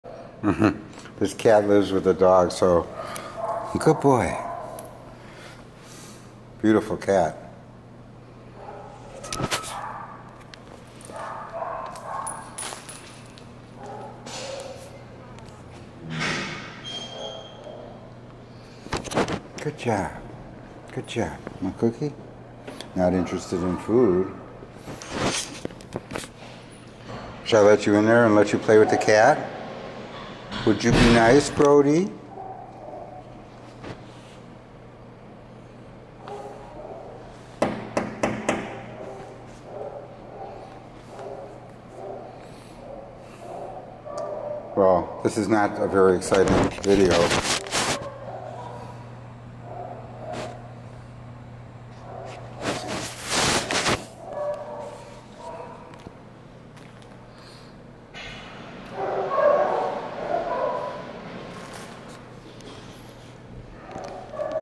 Allan Ross Dog Training
Brody N. is a 2.6 year old Boxer in returning Board and Train.  Here we practice obedience near a very playful cat in the lobby with distractions. 1/2